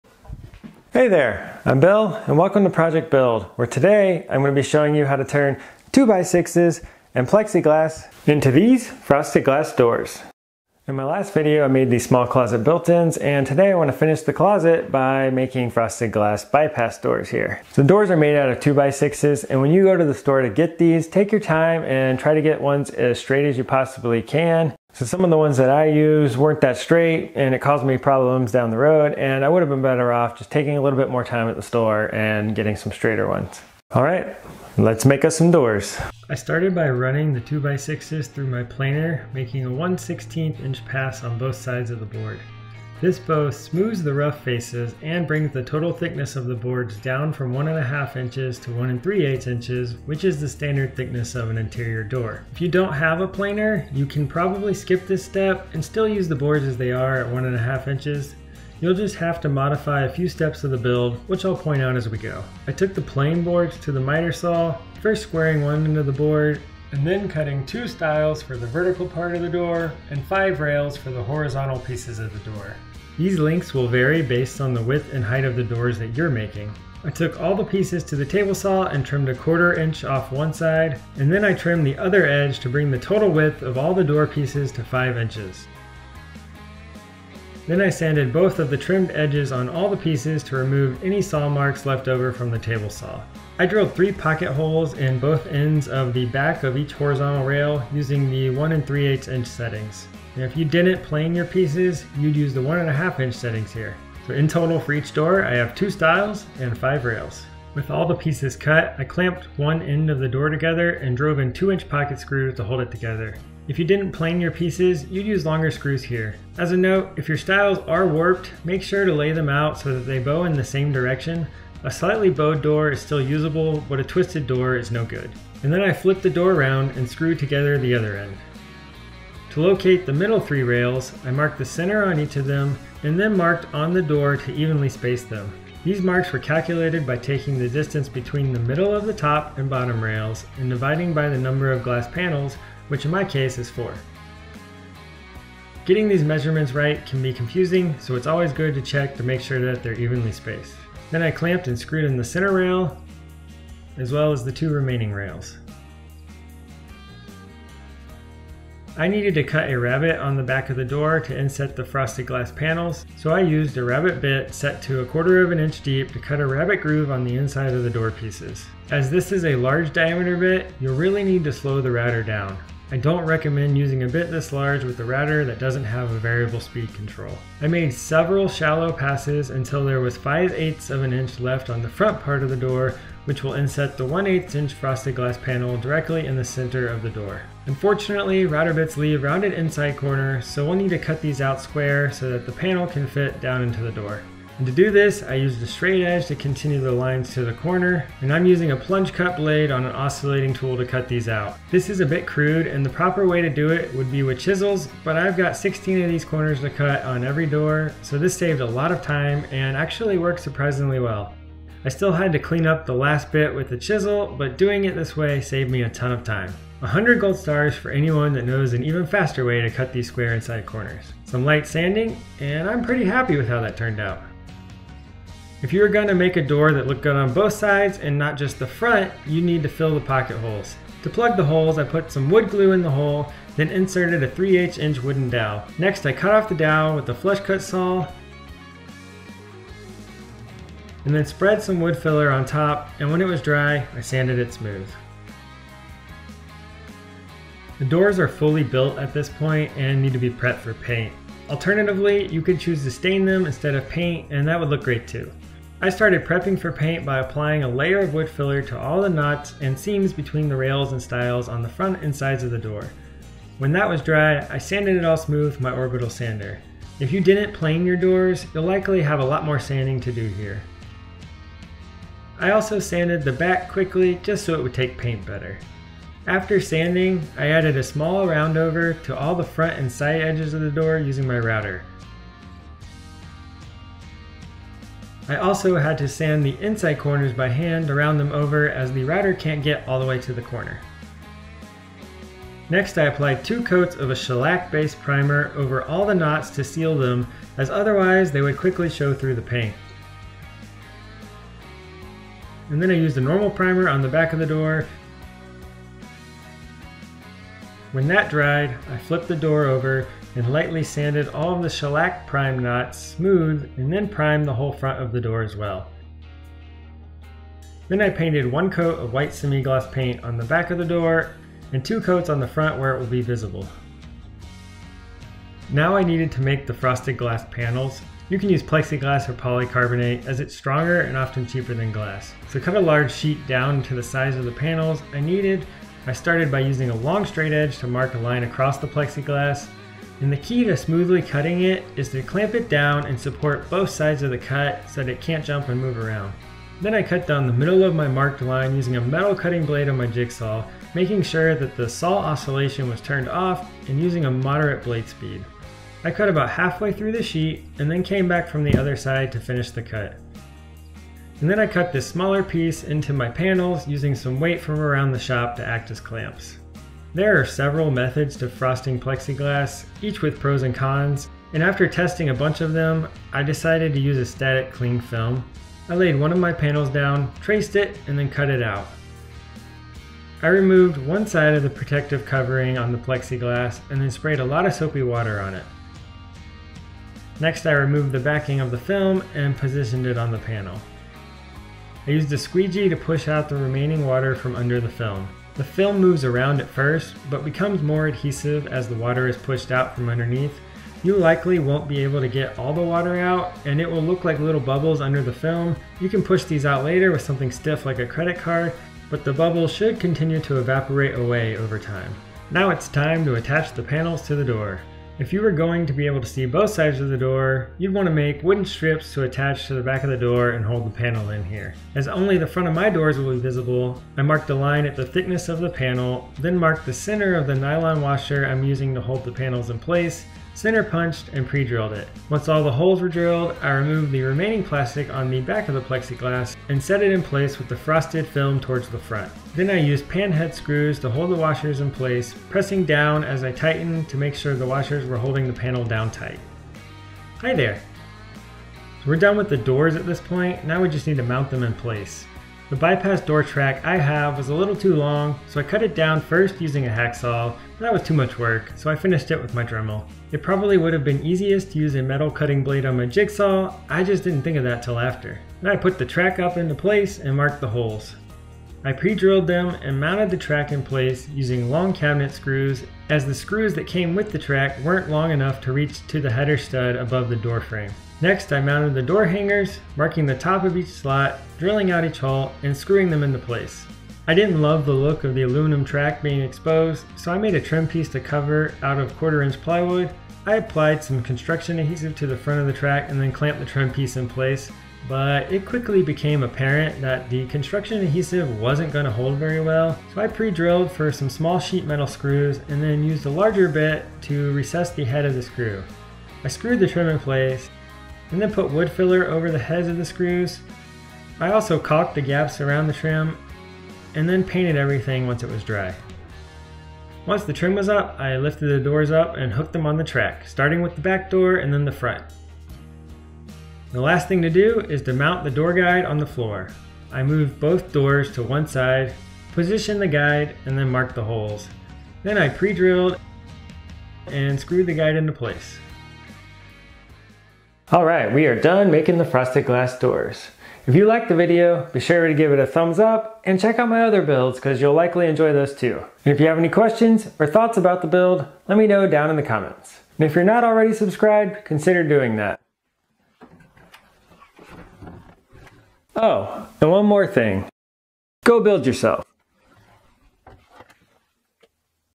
How to Make Frosted Glass Doors with 2x6s and Plexiglass
Learn how to make frosted glass doors with 2x6s and plexiglass using basic woodworking methods and common materials that you can buy from any home improvement store. I show you step by step how I made them so you can make them at home (and avoid my mistakes!)

Materials I used:
(most materials available at your local home improvement store)
2" Pocket Hole Screws
Wood Glue
Wood Filler
Stain Blocking Shellac Primer
Frosted Glass Film
1/16" Thick Nylon Washers
Bypass Door Hardware for 1 3/8" Doors
Bypass Hardware for 1 1/2" Doors

Tools I used:
(in order of appearance in the video)
Thickness Planer
Miter Saw
Table Saw
Magnetic Featherboard
Random Orbital Sander
Pocket Hole Jig
Portable Drill
24" F Clamps
Quick Grip Bar Clamps
Impact Driver
Measuring Tape
Handheld Router
Router Rabbeting Bit
Oscillating Multitool
Flush Cut Saw
Putty Knife
Router 1/16" Round Over Bit
Ratcheting Screwdriver

Chapters:
0:00 Intro
0:35 Making Door Rails and Stiles
1:47 Assembling the Door
4:52 Painting the Door
5:25 Making Frosted Plexiglass Panels
7:28 Installing Bypass Doors